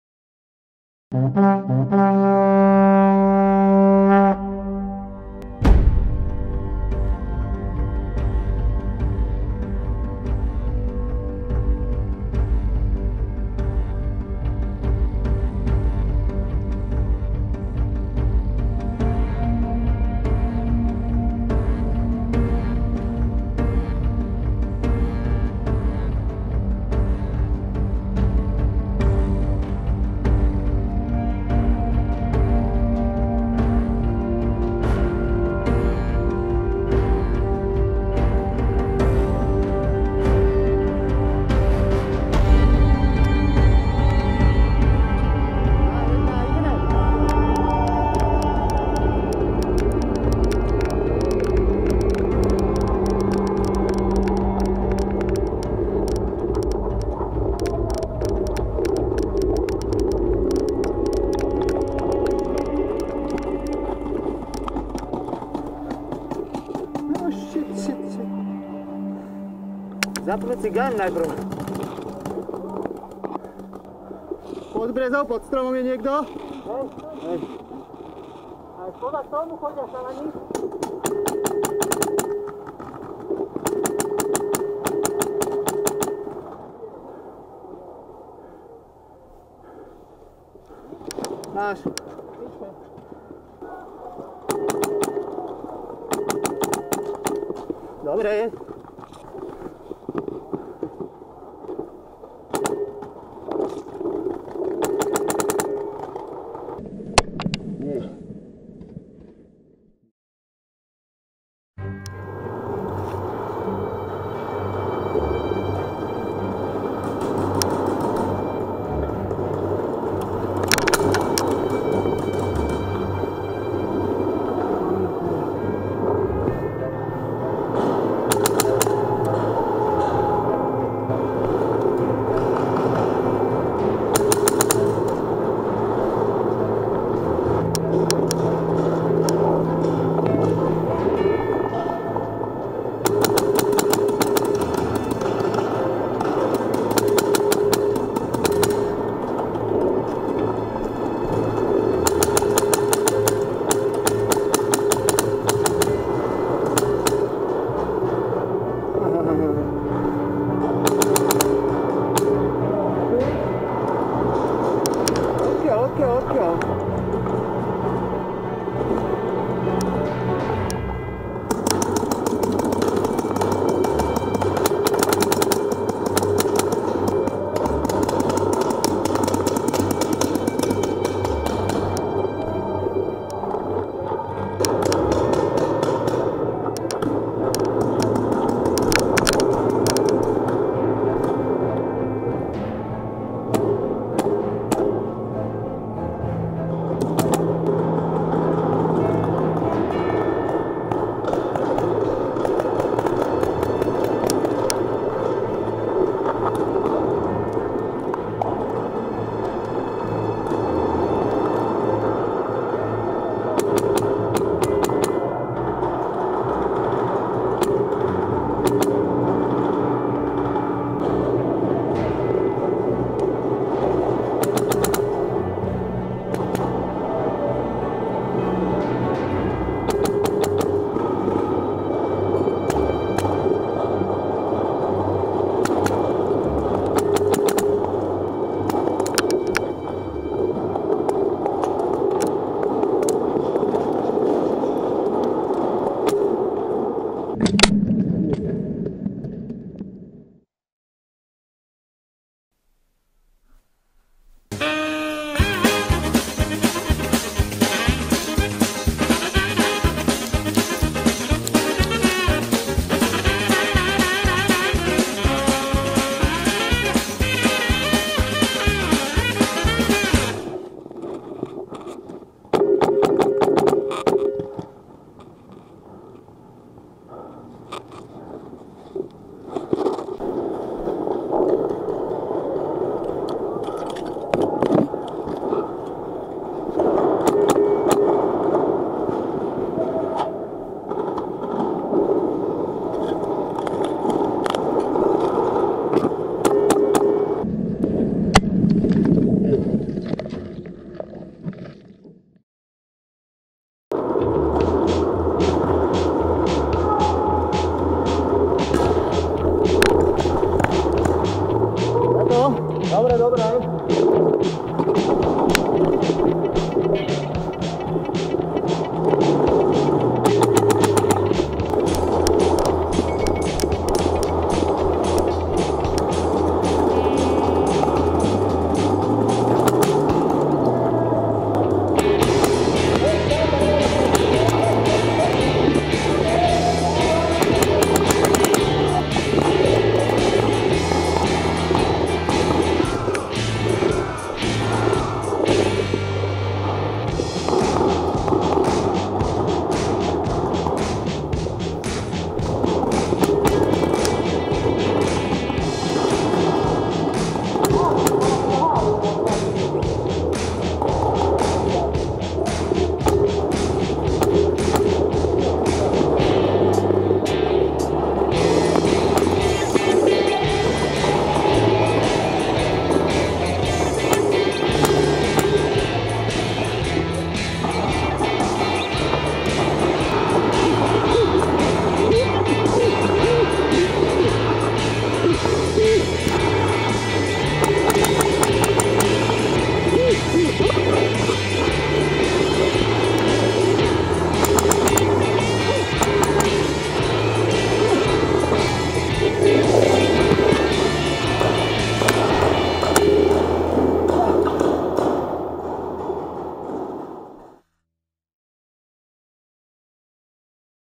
Solymar paintball game
Me, as Legio Draconus member, and guys from pbcentrum.sk were attending this event on 7th may of 2016 in Solymar, near Hungary capital city - Budapest.

The event was organised by Patriots paintball and Paintball zone... As usually, they did a great job. The cattering, paint and refs were top of the class :) so thanks very much